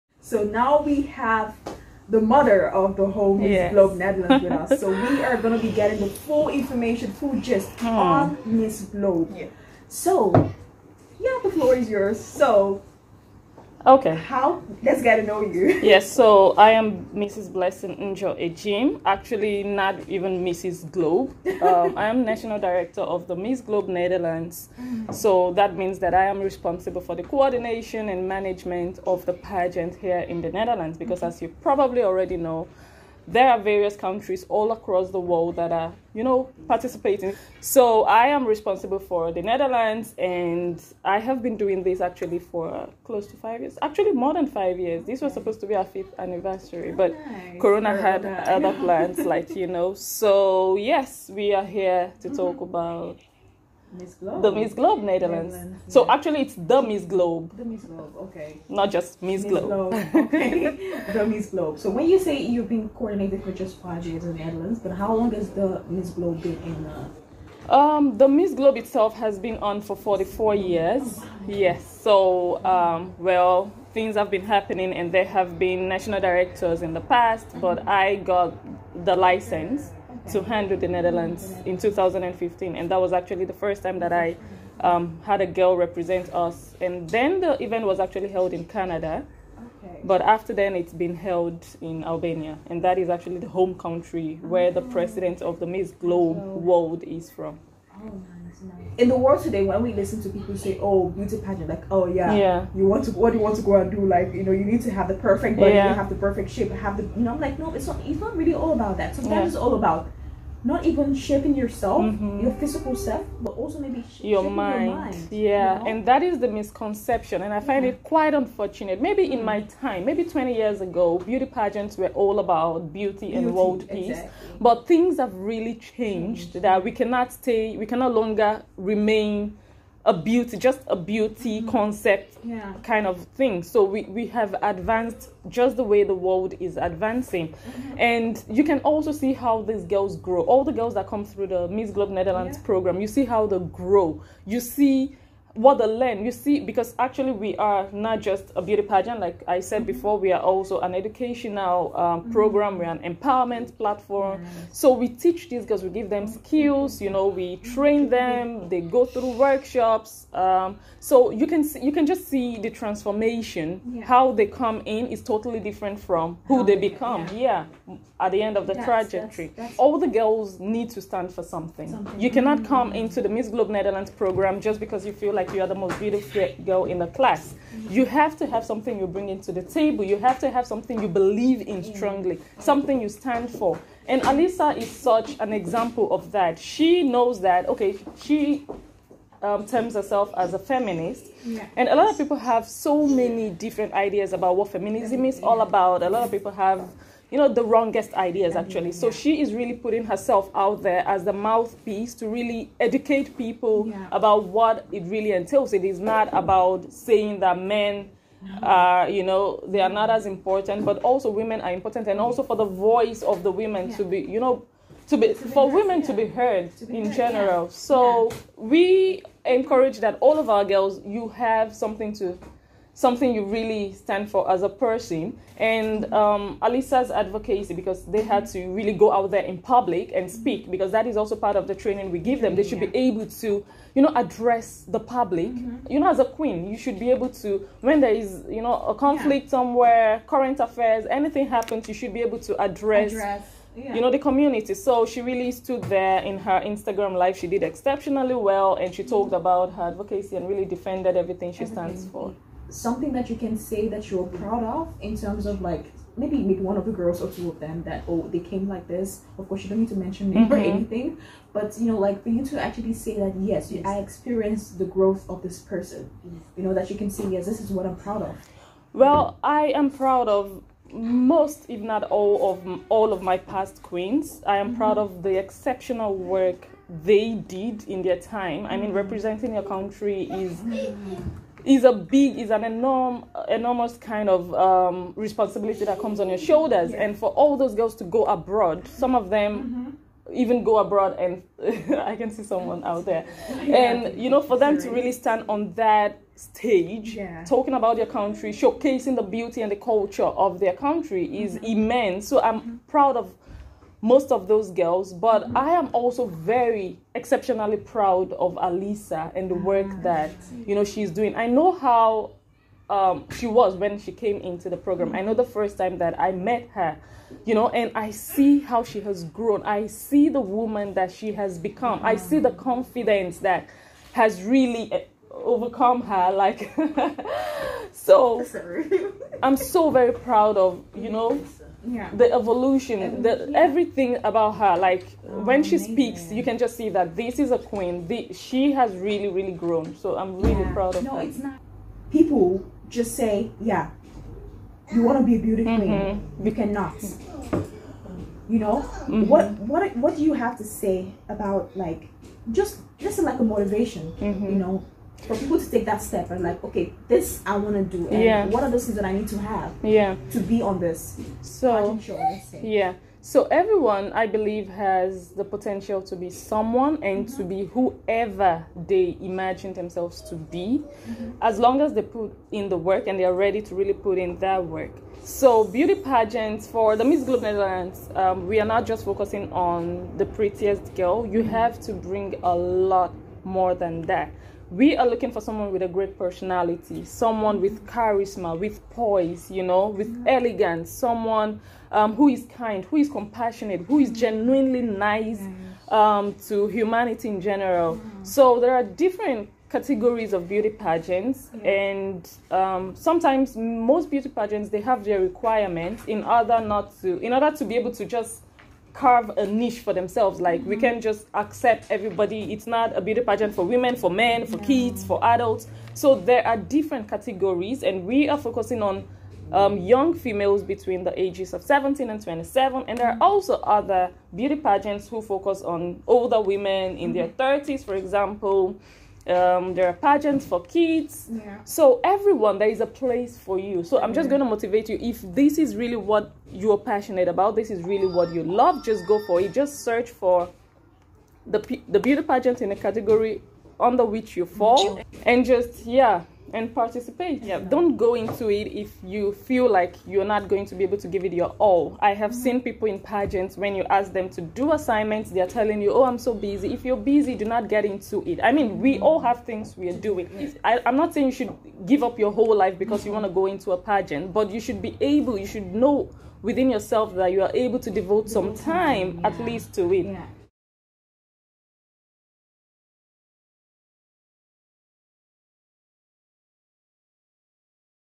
THE BEAUTY QUEEN MINDSET | NATIONAL DIRECTOR OF THE MISS GLOBE NETHERLANDS
Mrs. Blessing Njoh, National Director of the Miss Globe Netherlands, sat with 9JARADIO to talk about pageants, misconceptions, and the ultimate beauty queen mindset.